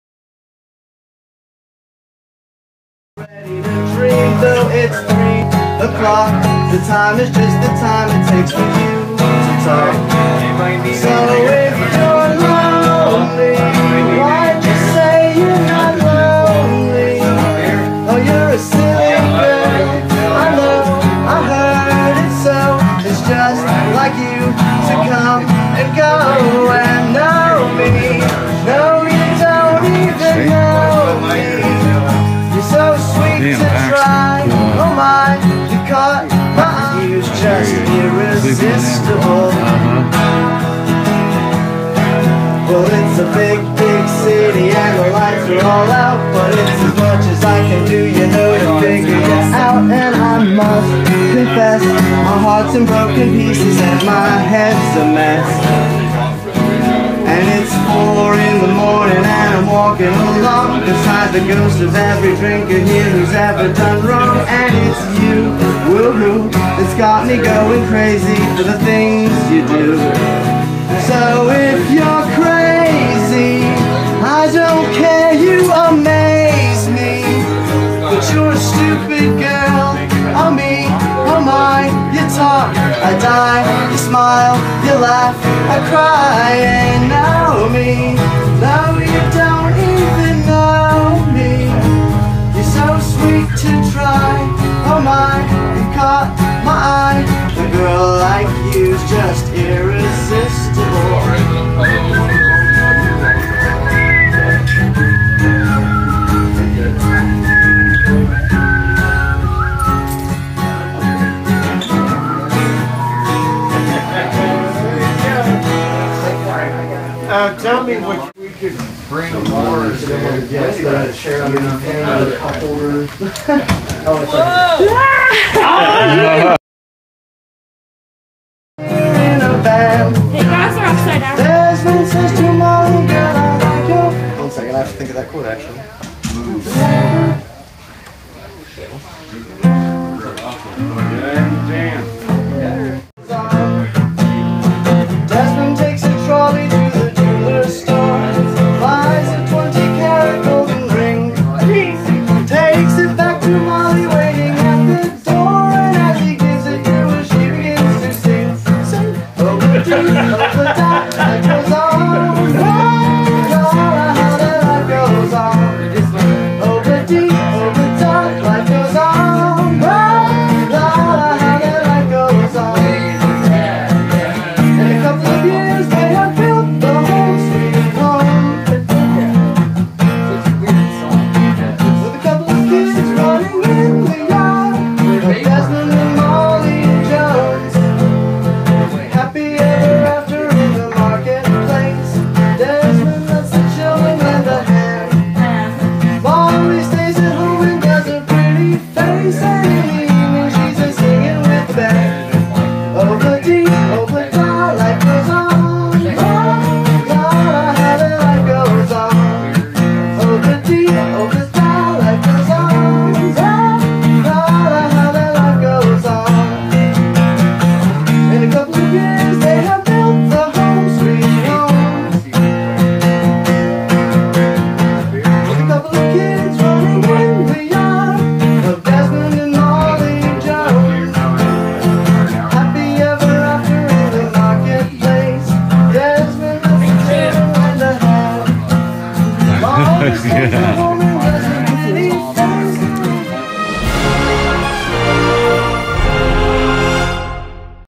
Vintage After Party 2
Another great WVCaresFEST After Party hosted by Vintage Restaurant.  Great vocals and guitar by a talented young musician, Brenden Setchell, was accompanied by a truly creative dance powered by grace, talent and glowing motivation.